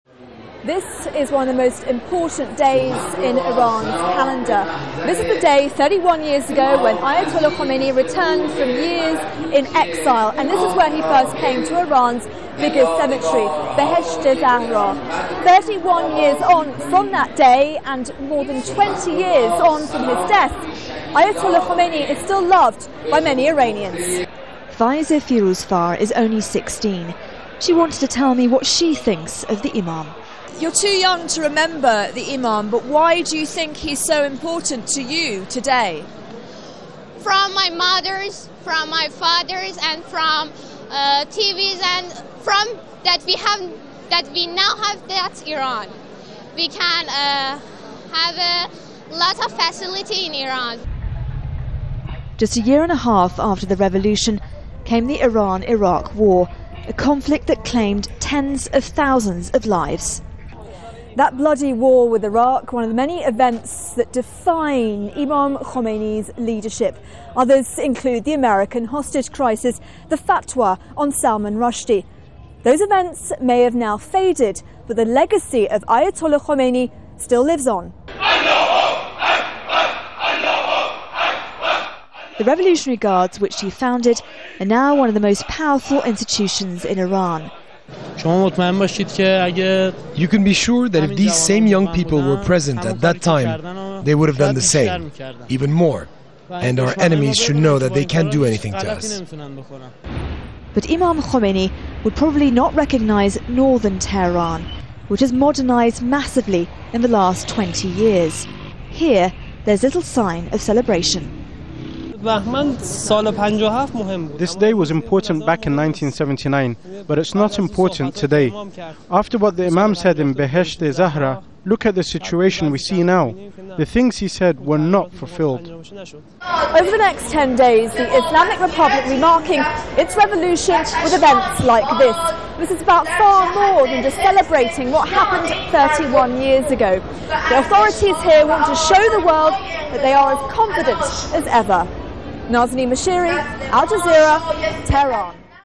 Iran marks anniversary of Khomeini's return
Iran is marking 31 years of the return of Ayatollah Ruhollah Khomeini to the country that triggered the Islamic Revolution.

Ten days of celebrations, known as the Fajr festival, are being held.

Hundreds of people gathered at the shrine of Khomeini, the leader of the revolution, on Monday.

Al Jazeera's Nazanine Moshiri reports from the cemetery in the capital, Tehran.